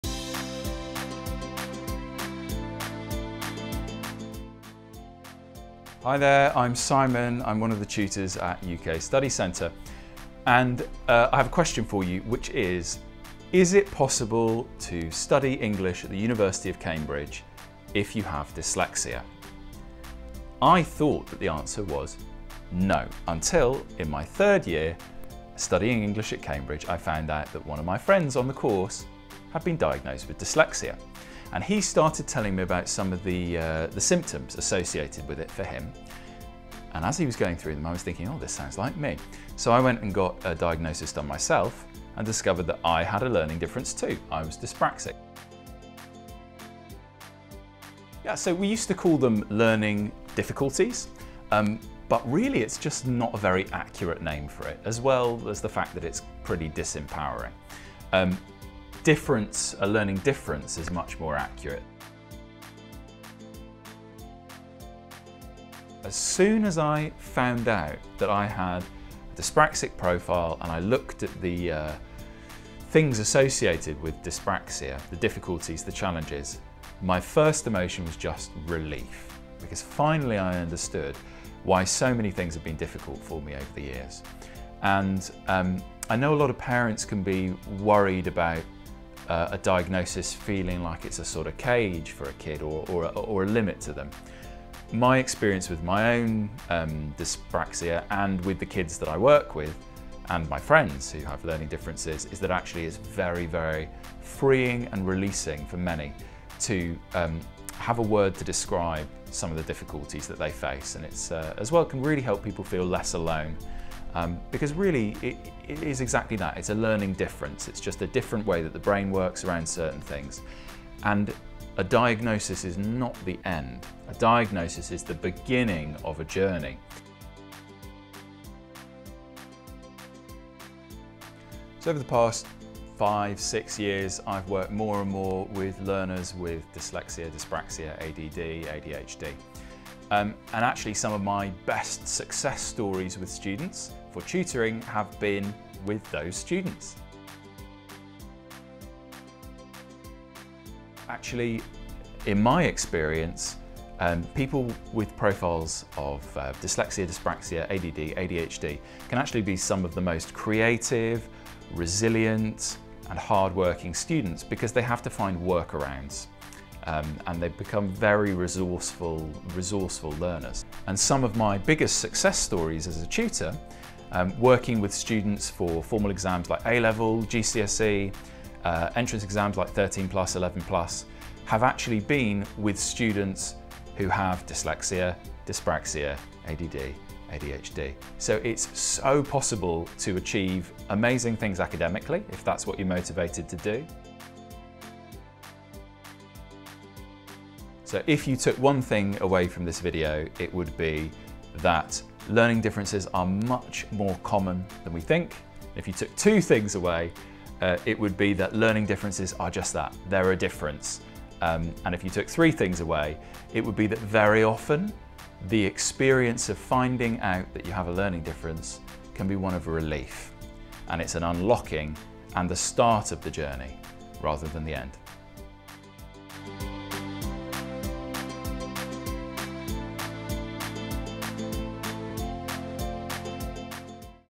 Let's Talk About Learning Differences with UKSC Tutor Simon | UK Study Centre
What are learning differences? Are you worried that a learning difference will hold your child back from achieving their full potential?

UK Study Centre tutor Simon shared his experience with dyspraxia, from his diagnosis to his work supporting students with learning differences.